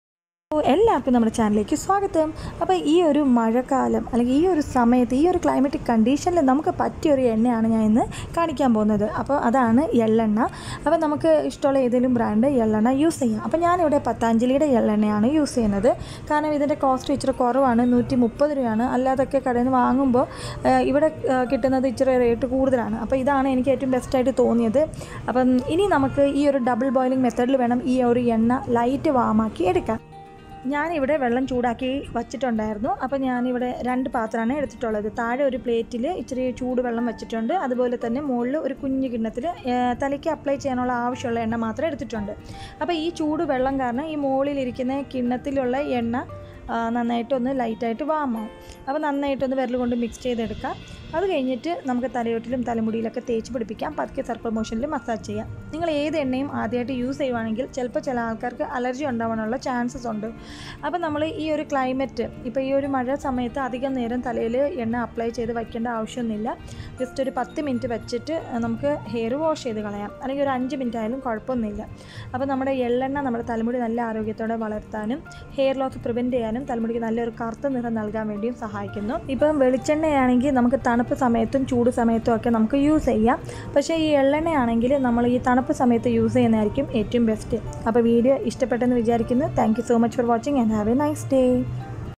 മഴക്കാലത്ത് എള്ളെണ്ണ ഉപയോഗിച്ച് തലമുടി നല്ല ആരോഗ്യത്തോടെ വളർത്തിയെടുക്കാം//PREVENT HAIR LOSS
Hello friends today am sharing how sesame oil helps to prevent premature grey hair abd hair loss and helps to grow hair faster.